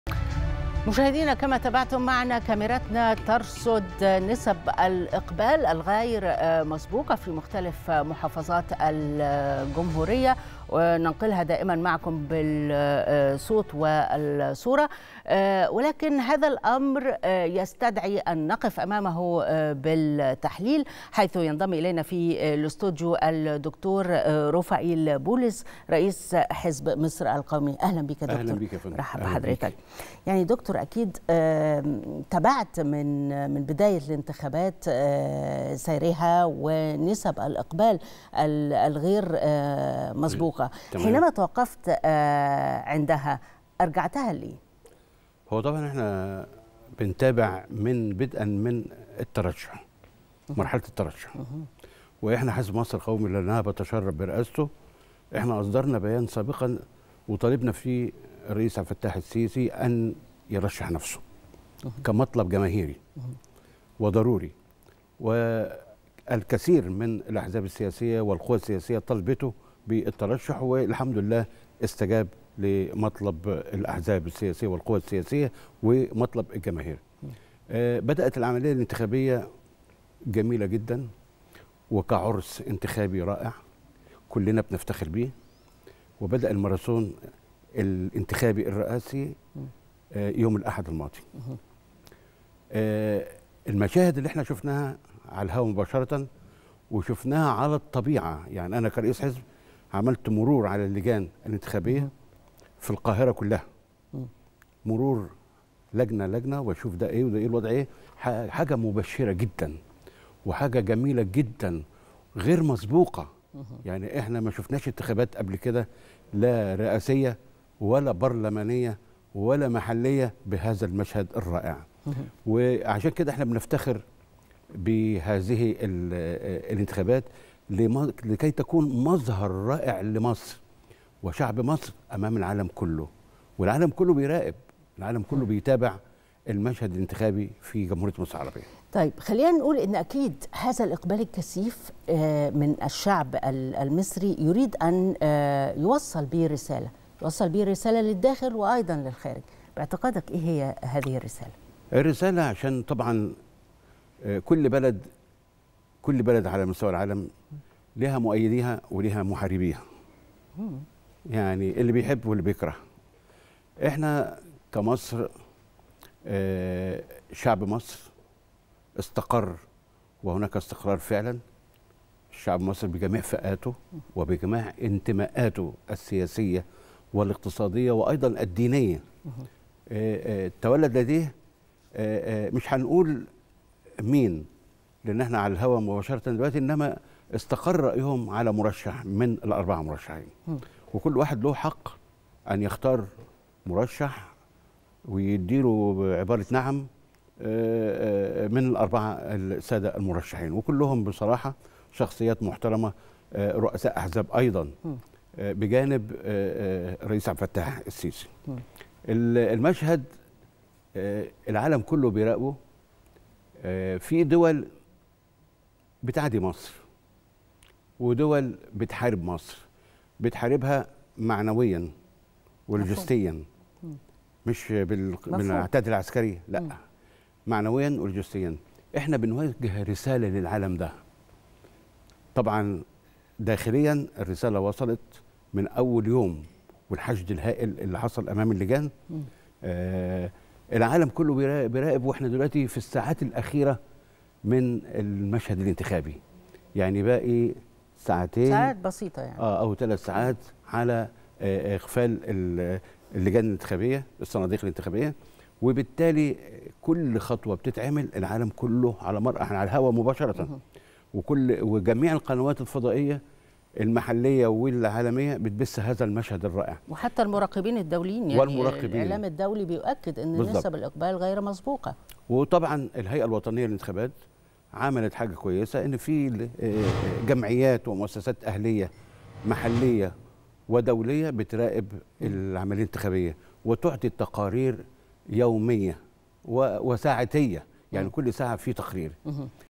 دروفائيل بولس:حزب مصر القومي اصدرنا بيان سابقا نطالب فيه الرئيس السيسي أن يرشح نفسه كمطلب جماهيري
د. روفائيل بولس: حزب مصر القومي اصدرنا بيان سابقا نطالب فيه الرئيس السيسي أن يرشح نفسه كمطلب جماهيري